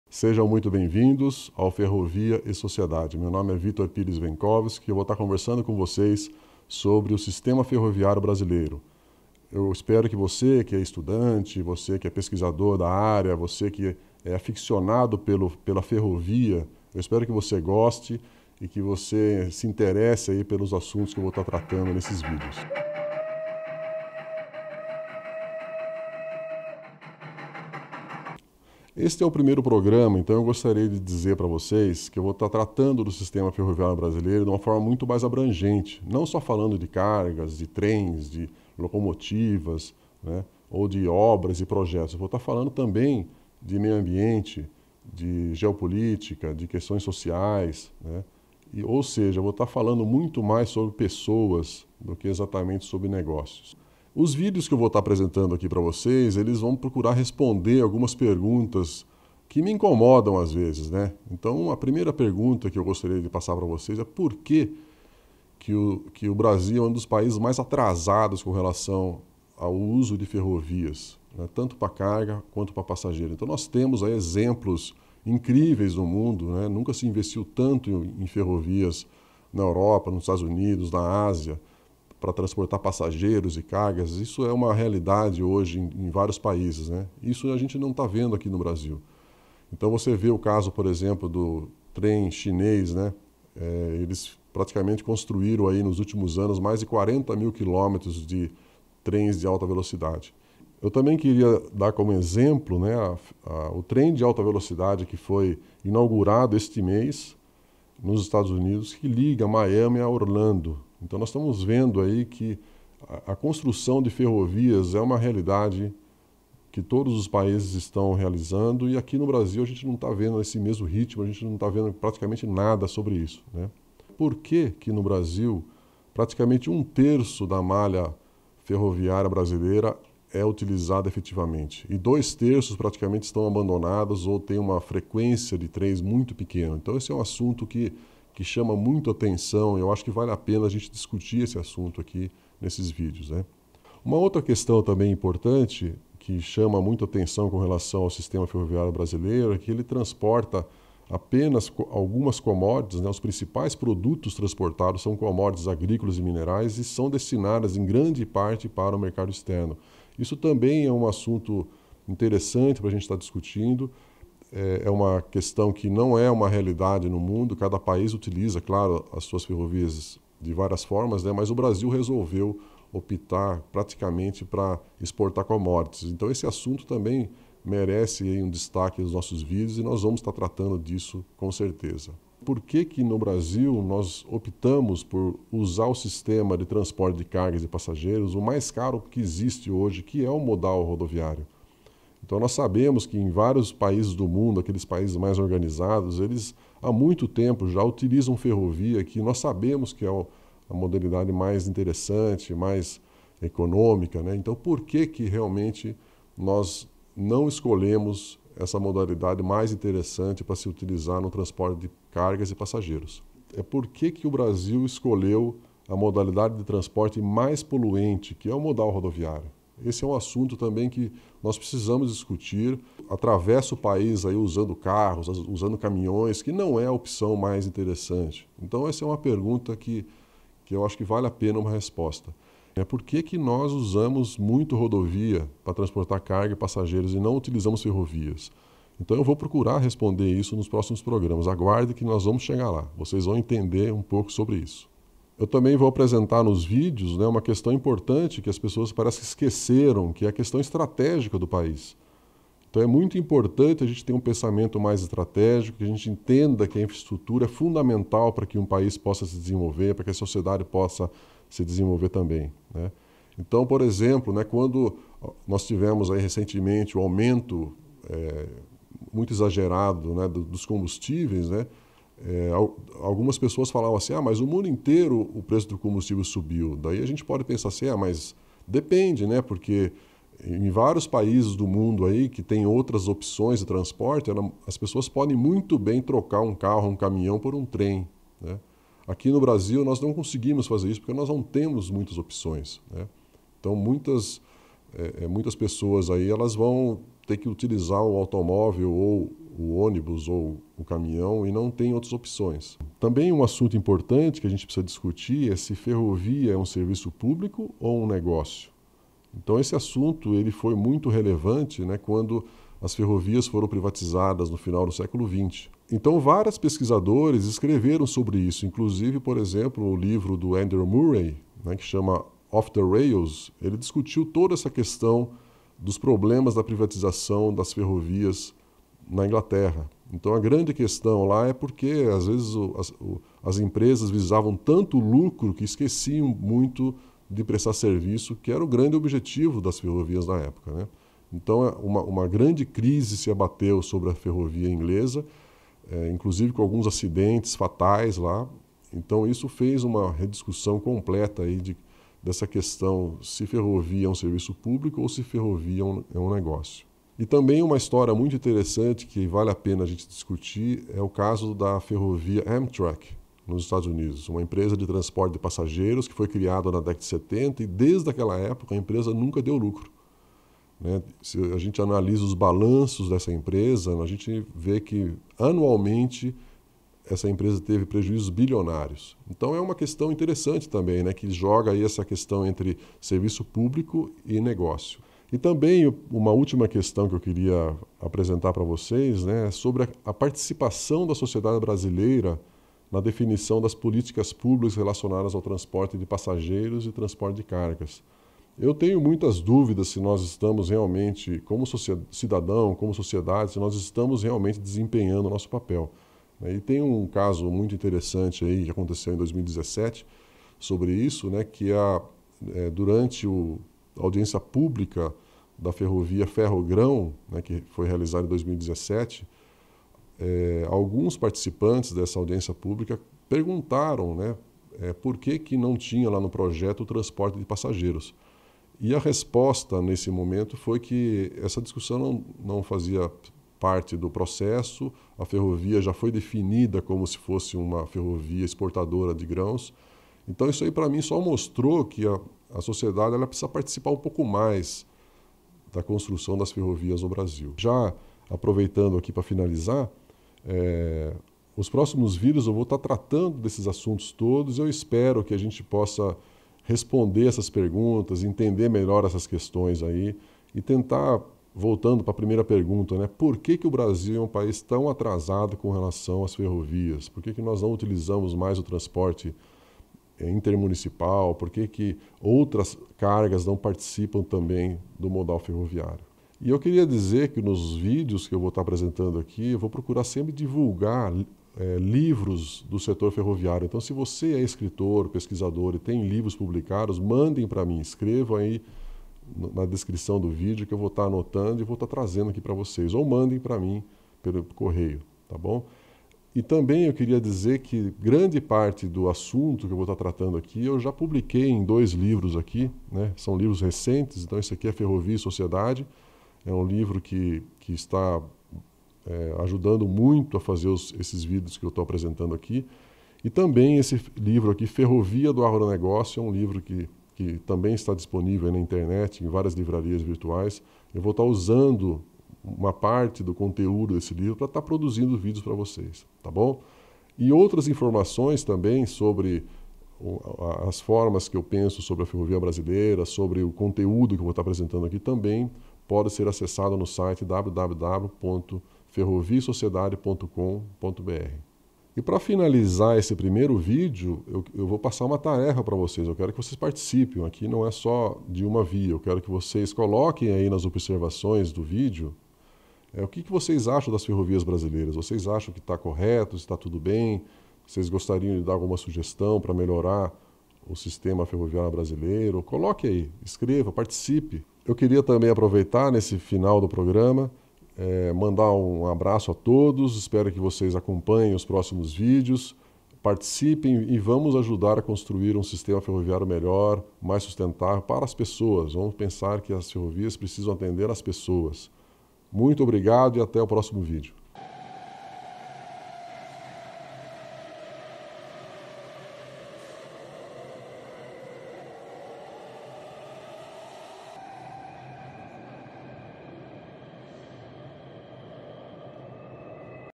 Assim caminham nossas ferrovias
Primeiro episódio da série "Ferrovia e Sociedade" #1

Livros indicados

- Ferrovia e Sociedade - Vitor Pires Vencovsky
- Ferrovia do agronegócio - Vitor Pires Vencovsky

Acesse outras informações - artigos e fotos

Introdução (0:00)
Abertura (00:23)
Abordagem do programa (0:28)
Perguntas que incomodam (0:54)
Exemplos no mundo (1:13)
Um terço da malha brasileira é utilizada (2:04)
Apenas commodities? (2:23)
Por que escolhemos rodovias? (3:02)
Por que escolhemos a opção mais poluente? (3:33)
E a questão estratégica do país? (4:05)
Ferrovia é um serviço ou um negócio? (5:02)
A história da Amtrak (5:57)
Participação da sociedade nas políticas públicas (6:25)
Por que o Brasil é tão atrasado com relação ao uso das ferrovias? (7:27)
Indicação de livro (7:56)
Atividade (9:01)
Agradecimentos (9:24)
Encerramento (9:44)

Pesquisa e apresentação
Vitor Pires Vencovsky